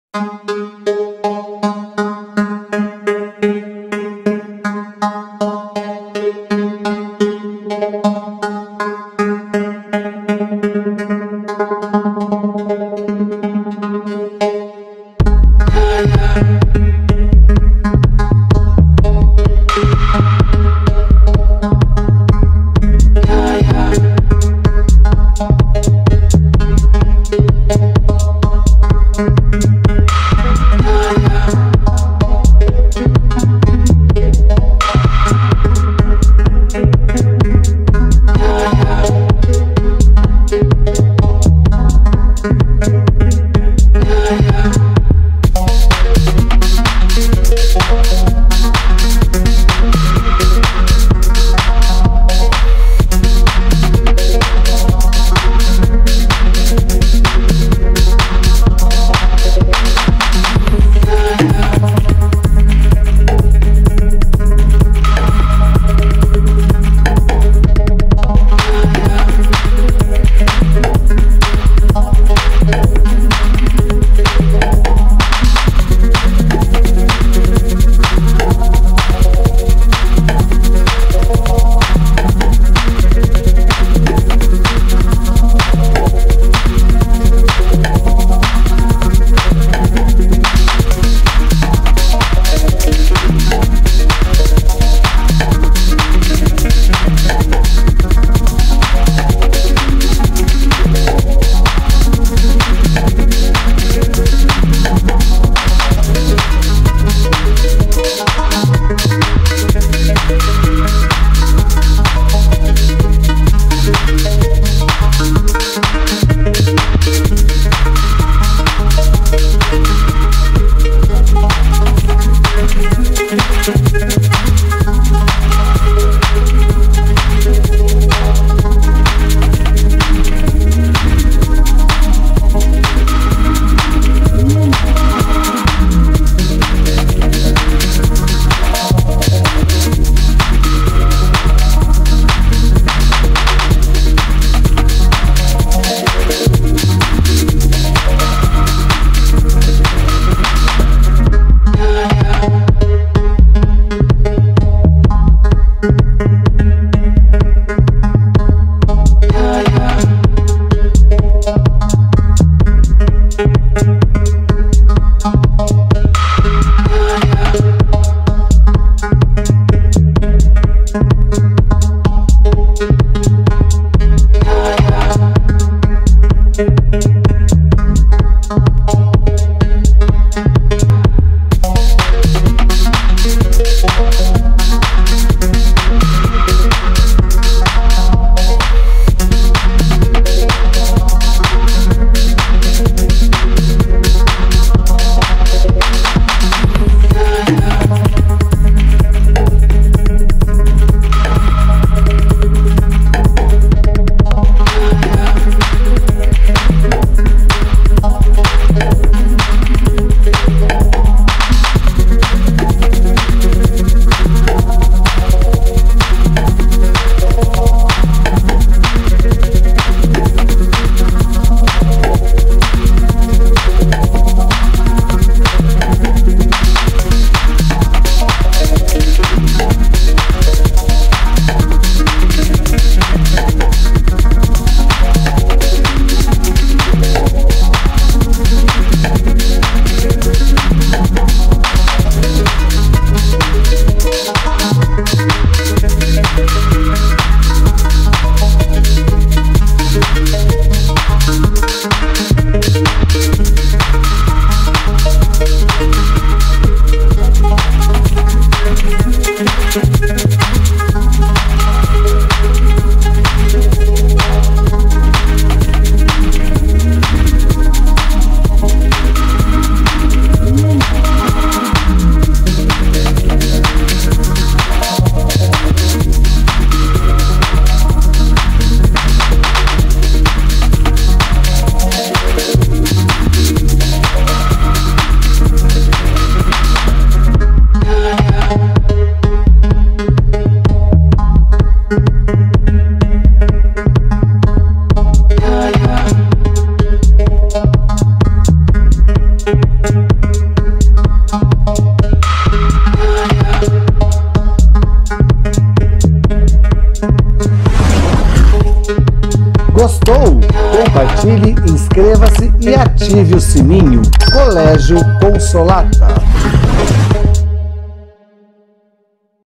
Colégio Consolata - Espaço Cultural 2021 -Ensino Médio - WorkCon -Tema - Medicamentos fitoterápicos!
Ensino Médio - WorkCon -Tema - Medicamentos fitoterápicos!

Colégio Consolata
Av. Imirim, 1424 - Imirim - São Paulo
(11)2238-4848